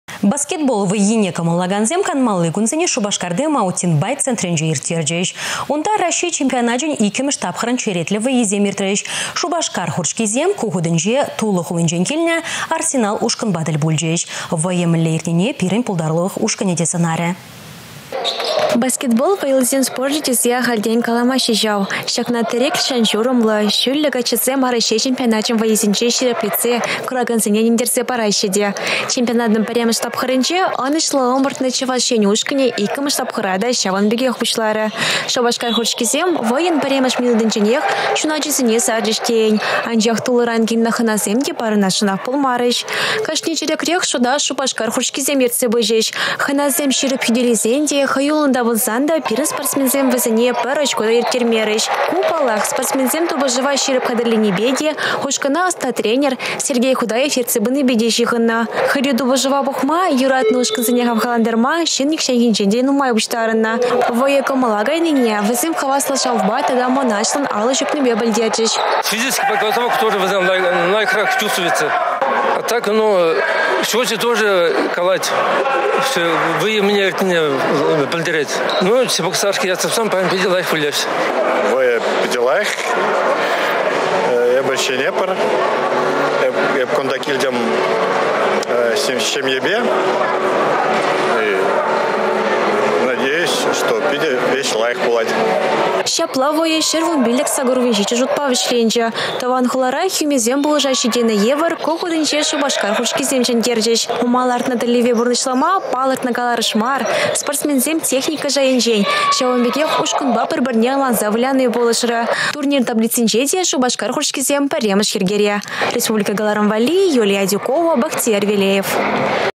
«Шупашкар хурчкисем» «Арсенал» ушкǎнпа тěл пулчěç
Баскетбол вǎййине кǎмǎллакансем канмалли кунсене Шупашкарти Маунтинбайк  центрěнче ирттерчěç. Унта Раççей чемпионачěн 2-мěш тапхǎрěн черетлě вǎййисем иртрěç. «Шупашкар хурчкисем» ку хутěнче Тула хулинчен килнě «Арсенал» ушкǎнпа тěл пулчěç. Вǎйǎ мěнле иртнине пирěн пултарулǎх ушкǎнě те сǎнарě.